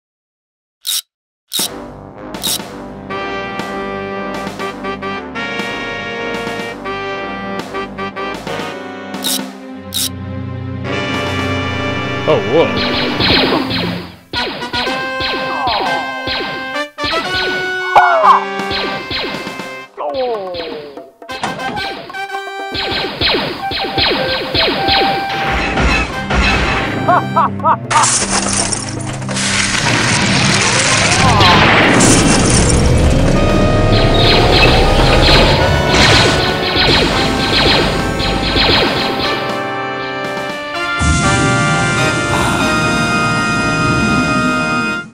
Stormtroopers must stop the Millennium Falcon
Footage I found and re-edited from 208-2009. Can they Surprise Han Solo!